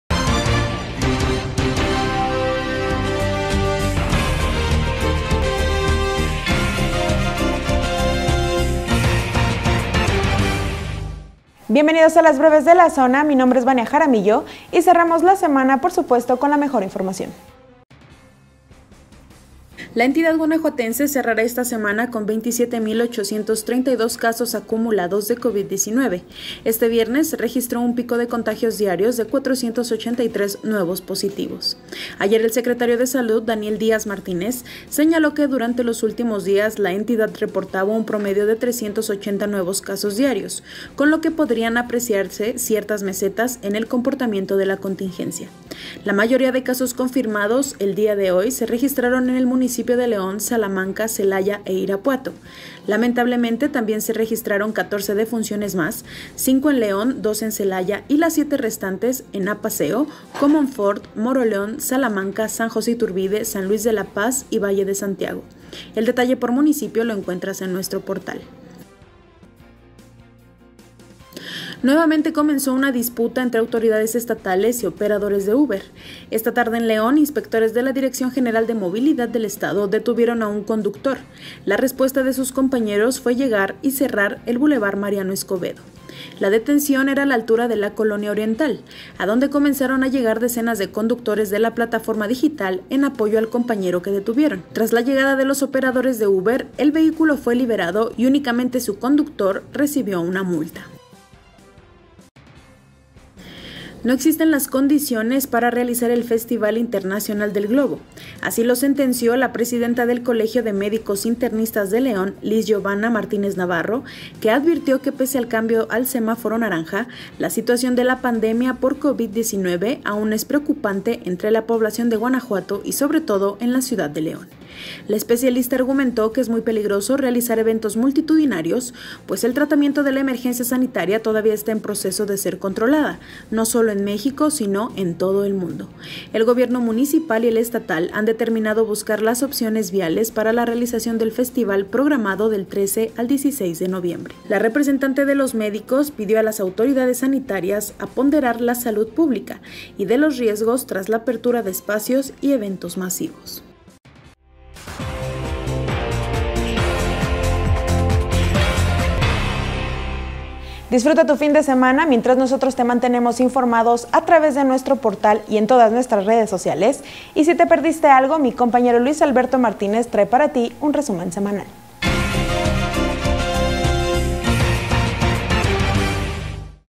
Cierra Guanajuato semana con 27 mil 832 casos totales de COVID-19 | Breves de la Zona
BrevesDeLaZona📰 | Este es el corte informativo de la noche
- Médicos piden reconsiderar realización del FIG; eventos multitudinarios son peligrosos.
- Protestan conductores de plataforma ejecutiva tras la detención de uno de sus compañeros, en León.
- Cierra Guanajuato semana con 27 mil 832 casos acumulados de coronavirus.